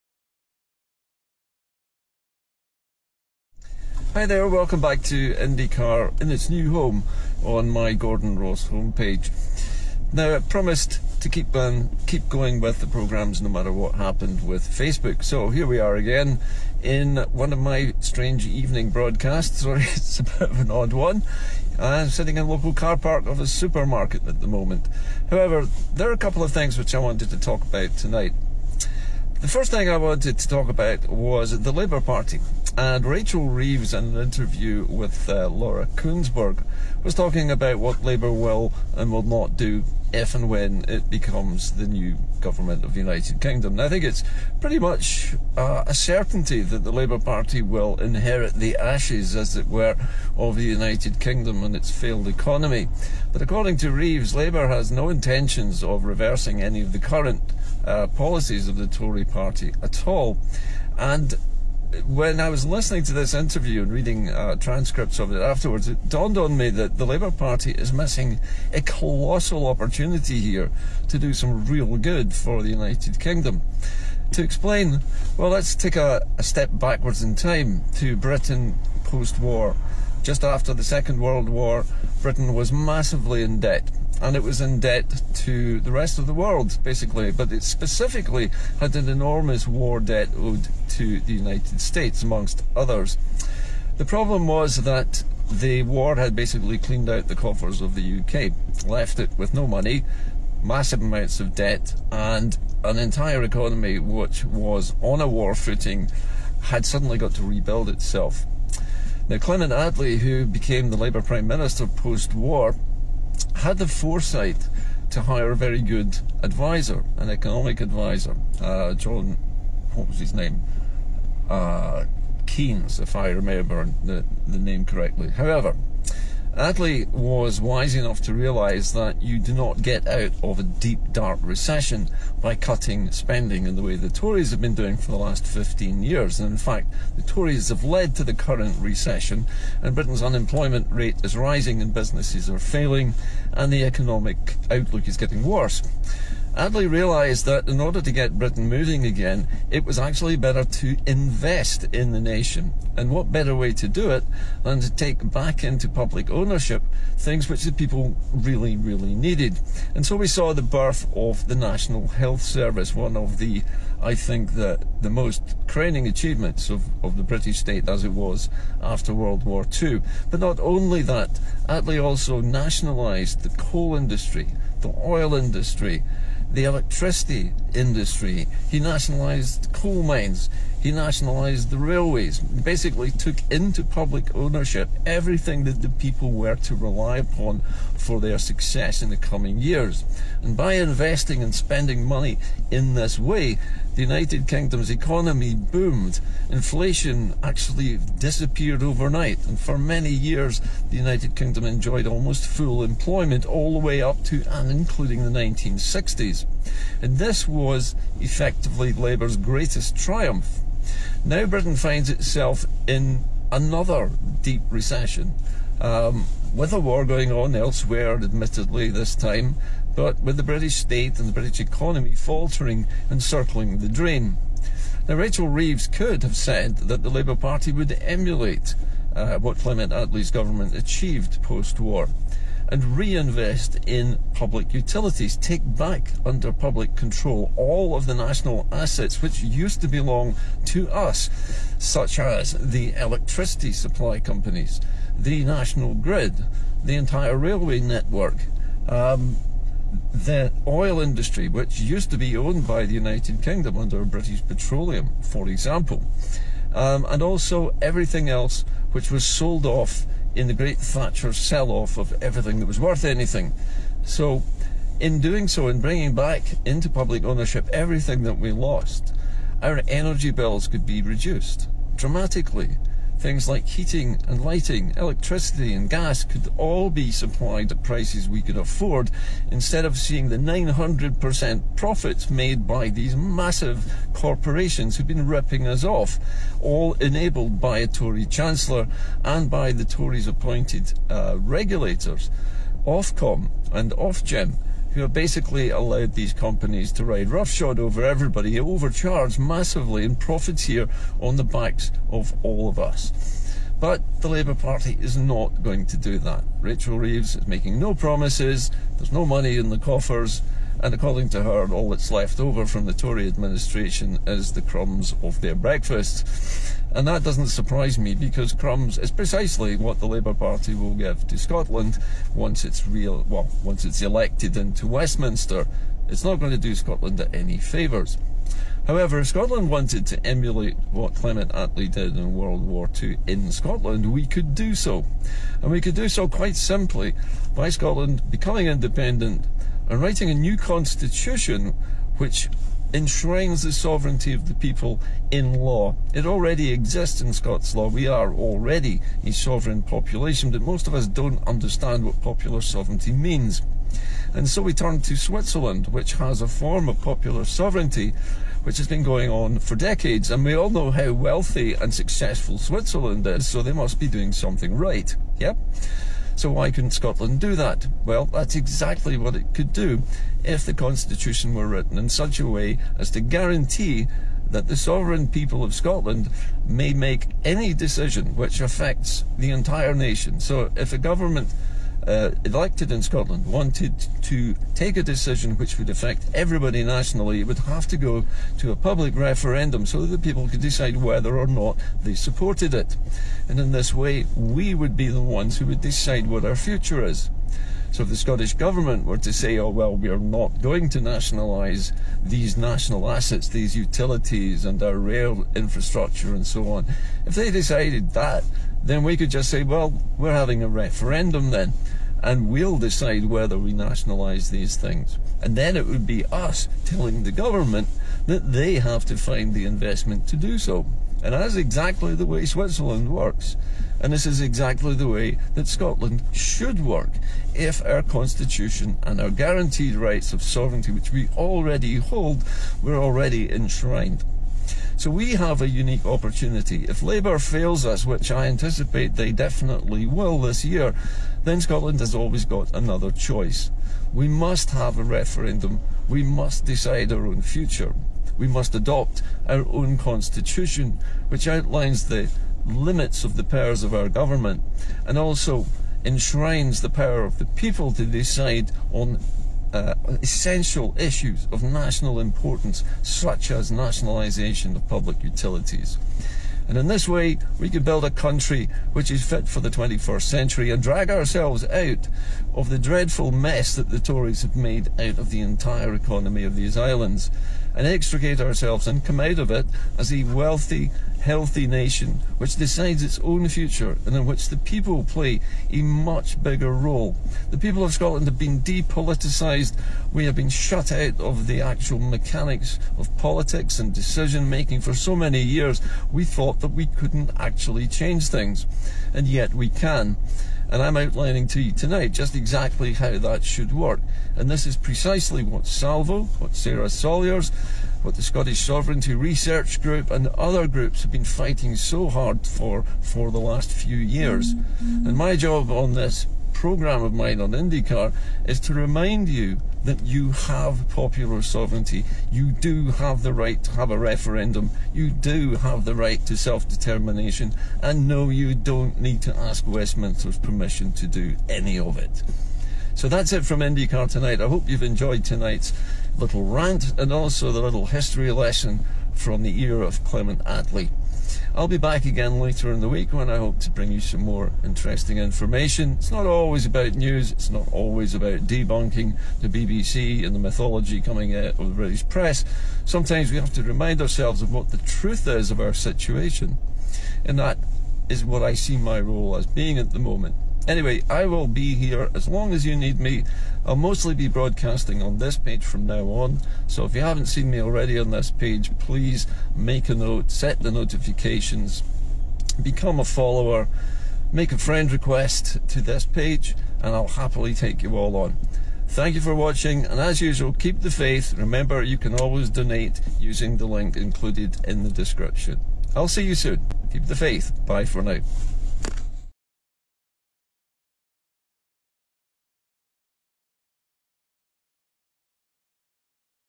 According to Rachel Reeves, labour will not renationalised transport energy or other privatised UK
🏴󠁧󠁢󠁳󠁣󠁴󠁿  IndyCar Gordon Ross -  12.3.24  🏴󠁧󠁢󠁳󠁣󠁴󠁿

The labour party is in a position to emulate the remarkable post war recovery enacted by Clement Atlees labour government. But according to Rachel Reeves, labour will not renationalised transport energy or other privatised UK national public utilities.

Scotland, with a constitution guaranteeing popular Sovereignty could emulate Switzerland and have public referenda to decide crucial national policies, such as nationalisation of big essential utilities and transport.